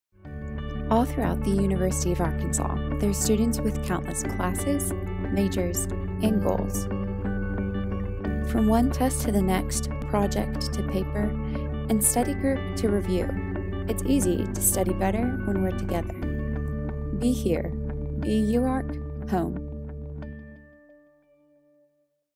Live on Campus This Fall
We still have room for you on campus at the University of Arkansas. Be here. Be UARKHOME.